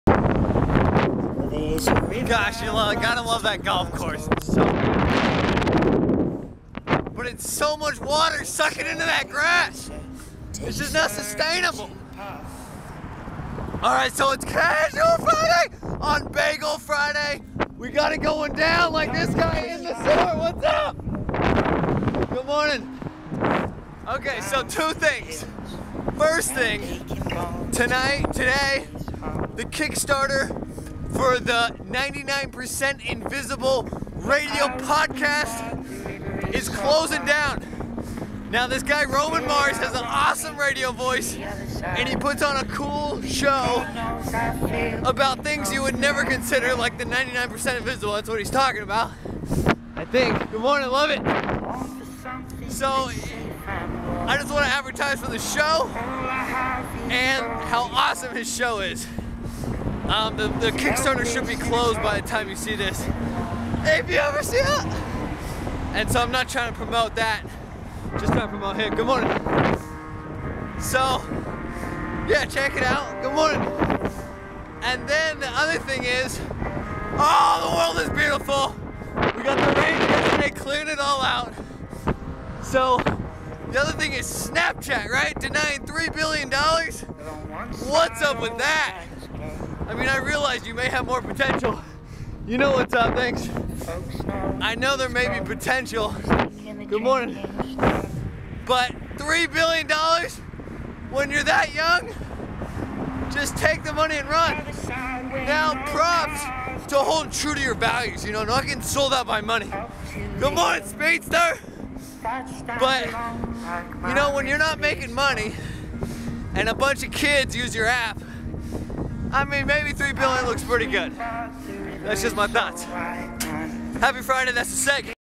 Rambling on a Bike - 99% Invisible Kickstarter Project, SnapChat not selling out to Facebook for $3B
Honestly such a great intersting show to listen to. Roman has a great radio voice, and here are some of my top three episodes listened to thus far:

Maybe they know what they are doing, but $3B is alot of money for an app with Zero monetization currently. My Top Bike Products:

My Top Bike Tools For My Garage:

23 Piece Bike Tool Kit